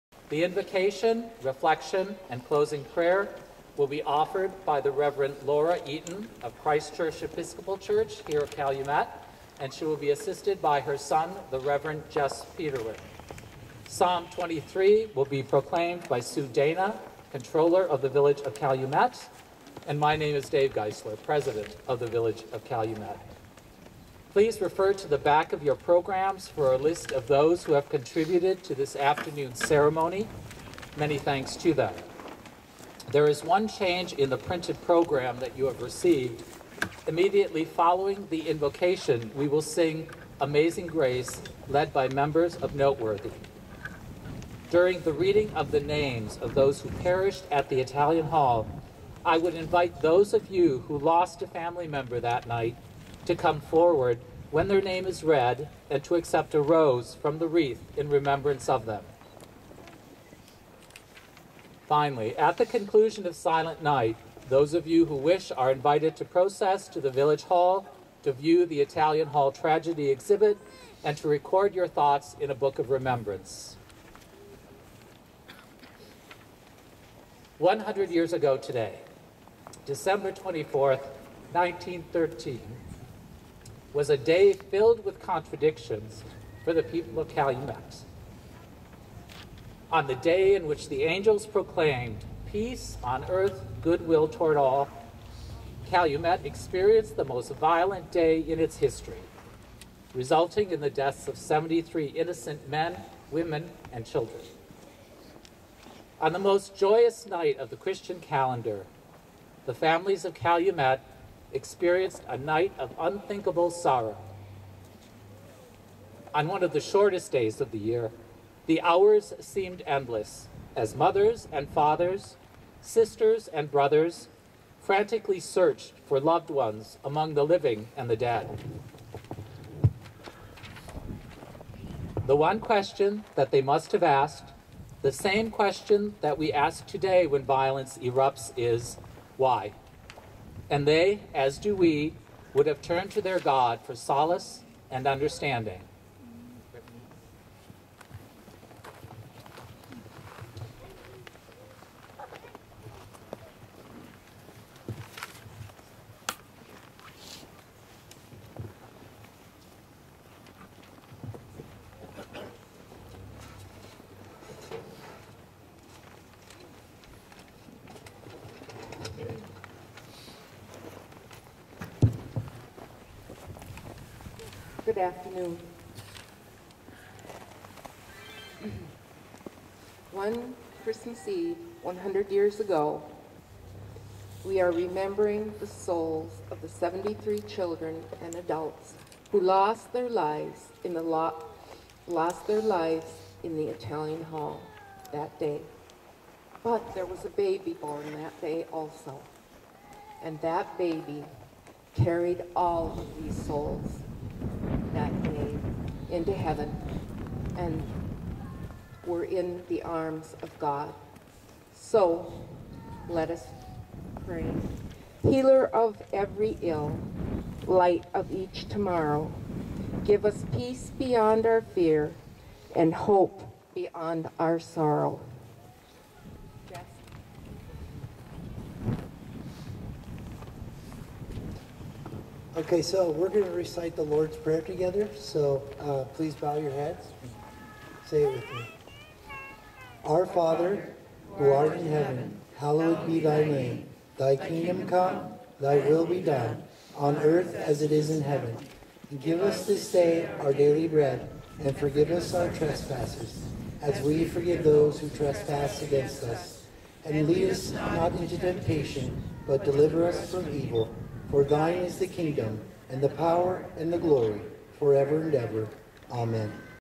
1 ital hall ceremony dec24
On Dec. 24, 2013, the village of Calumet, Mich., commemorates the centennial of the Italian Hall disaster, when, during a Christmas party for striking miners' children, 73 people, mostly children, perished after someone yelled a false alarm of "Fire." There was no fire, but the victims were crushed in a stairwell as they tried to escape the building. David Geisler, Calumet Village president, welcomes residents and visitors to the commemoration and introduces the other speakers. Luminaries and a wreath with roses honor the victims. An arch, which is all that remains of the Italian Hall, is a historic site that commemorates the tragedy. (Video by Allan Baker for Keweeenaw Now)